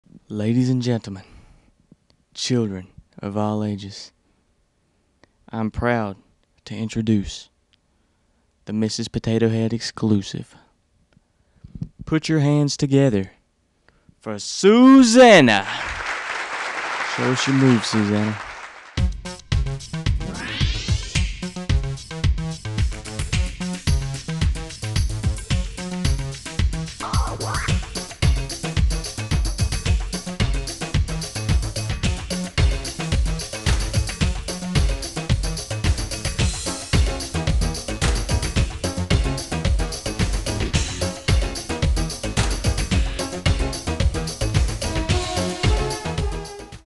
Mrs. Potato Head Dances
A stop motion video produced by Chase High School students in the TV performance class at Isothermal Community College.